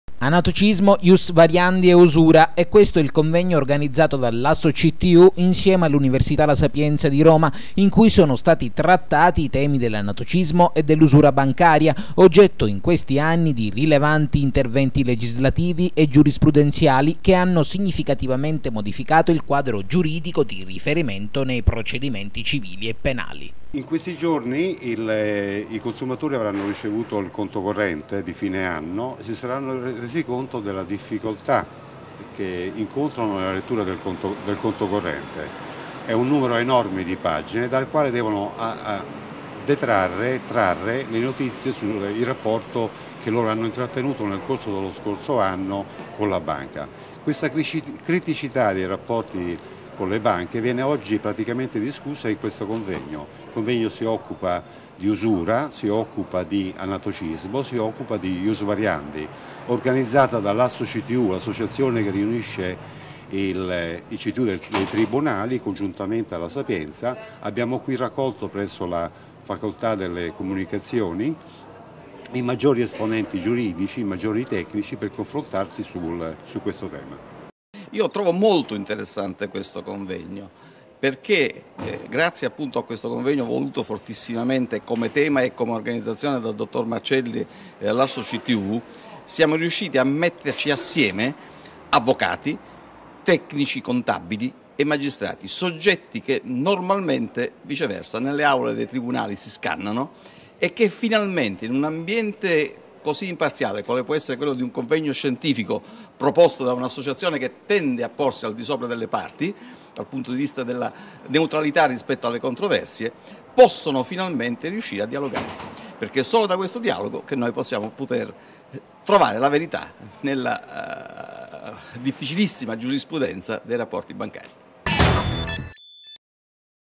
"ANATOCISMO, IUS VARIANDI E USURA"
"Anatocismo, ius variandi e usura", è questo il convegno organizzato dall'Assoctu insieme all'Università La Sapienza in cui sono stati trattami i temi dell'anatocismo e dell'usura Bancaria, oggetto in questi ultimi anni di rilevanti interventi legislativi e giurisprudenziali che hanno significativamente modificato il quadro giuridico di riferimento nei procedimenti civili e penali.

(seguono interviste al Dott. Roberto Marcelli -- Presidente ASSOCTU; Prof. Paolo Grassi - Università di Teramo)